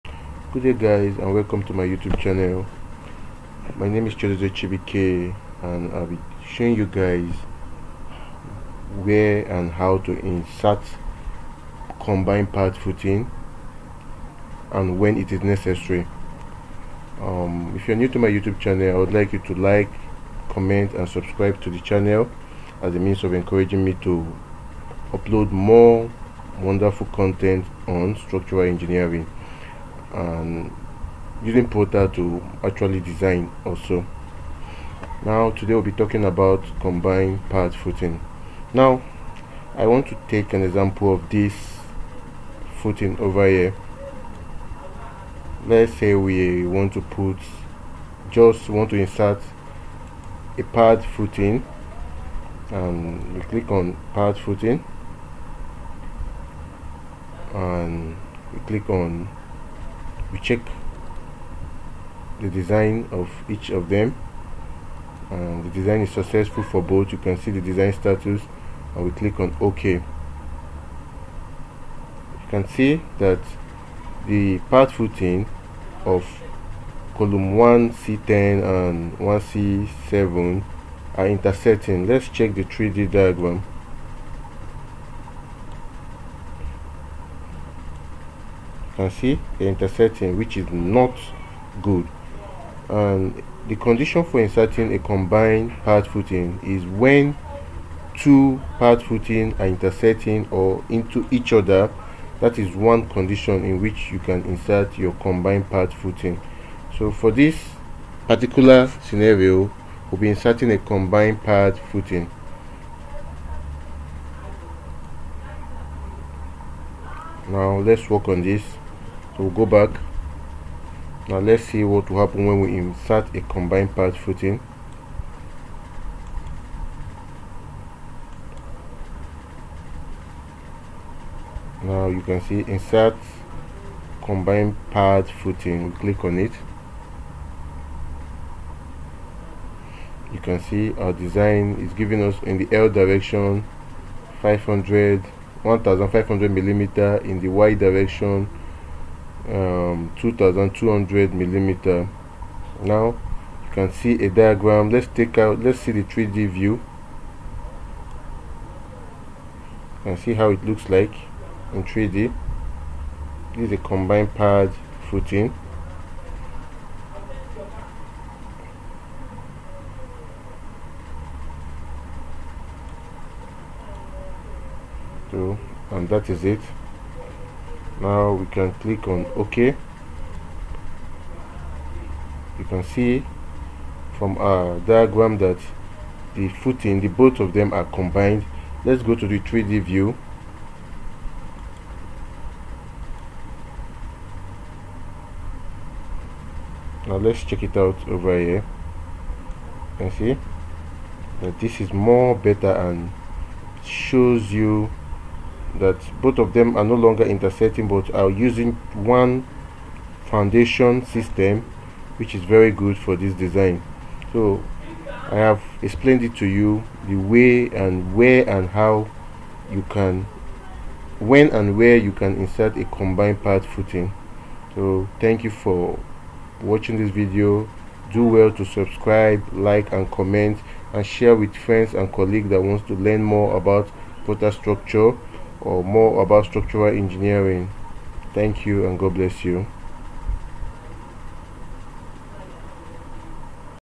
HOW AND WHEN TO USE A COMBINED PAD FOOTING IN PROTA STRUCTURE
This video shows the viewer the condition necessary to use a combined pad footing and step in designing a combined pad footing in Prota Structure.